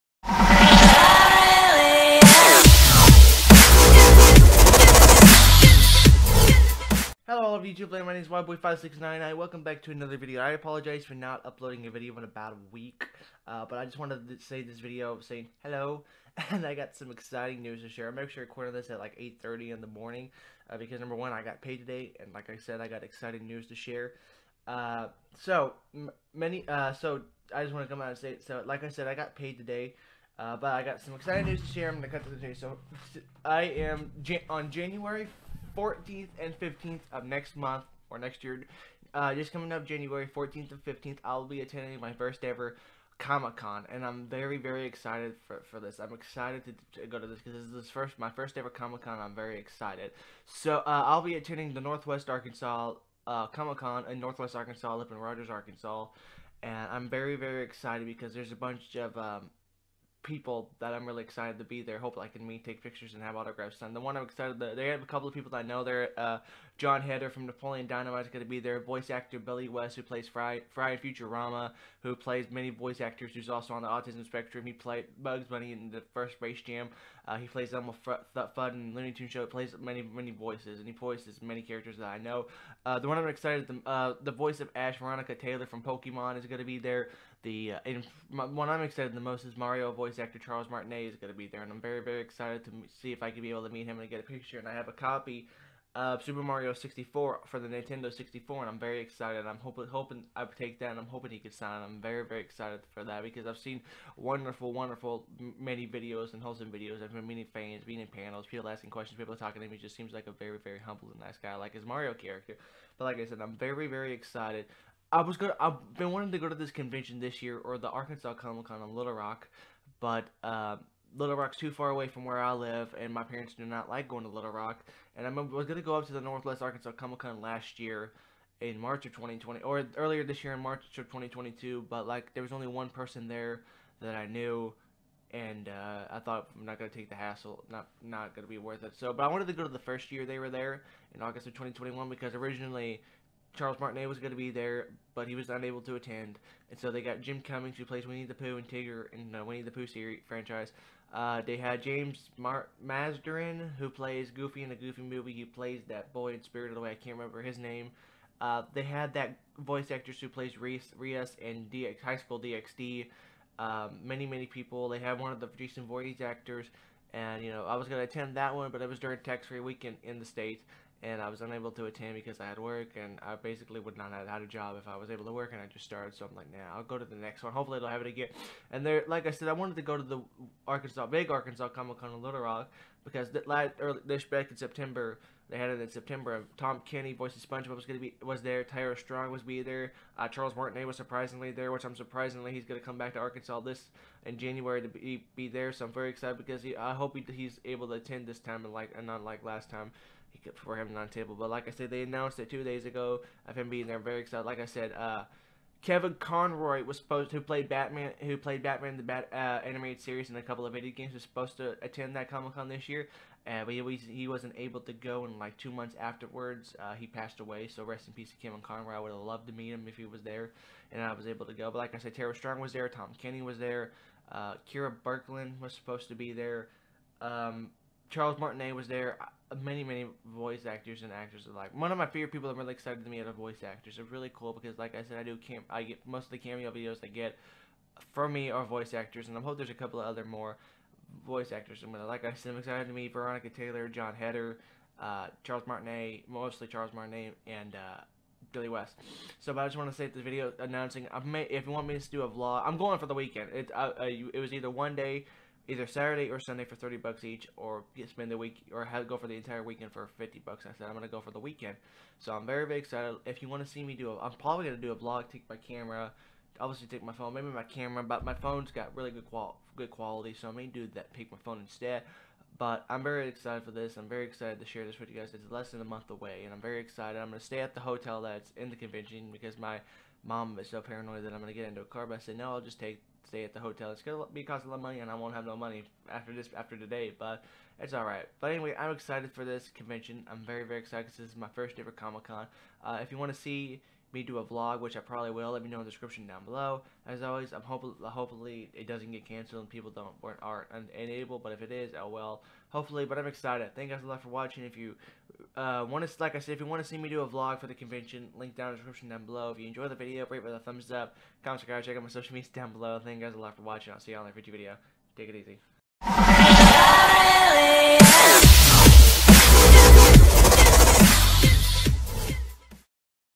I'M ATTENDING MY FIRST COMIC CON IN JAURANY 2023
I will be attending my first ever comic con called The Northwest Arkansas Comic Con in Rogers, Arkansas on January 14th and 15th, 2023. I am so excited!

► Feel free to send me a letter!
Wildboy5699
Lavaca, Arkansas 72941